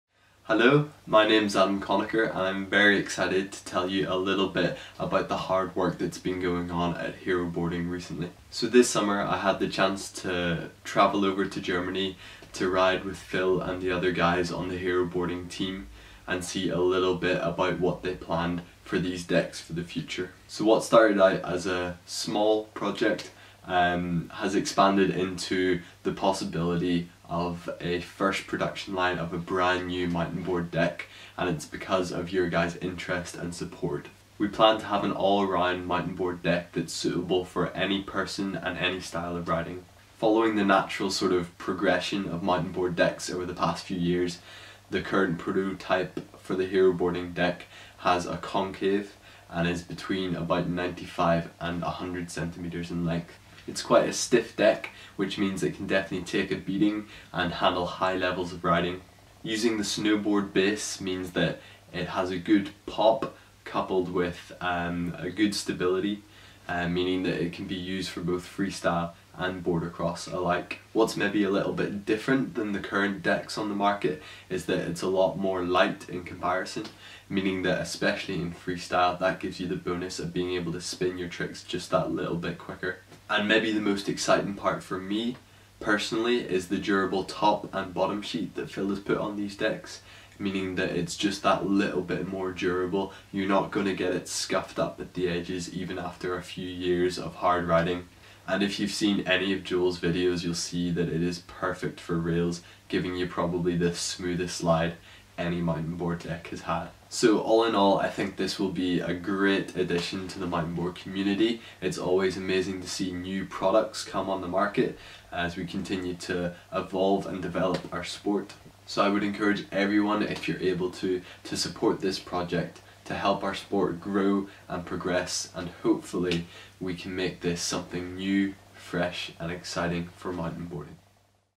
Haero Boards Bro Model Testimony
Hear what Adam Conacher, the first one to test our prototypes, have to say about the Bro Model and our project!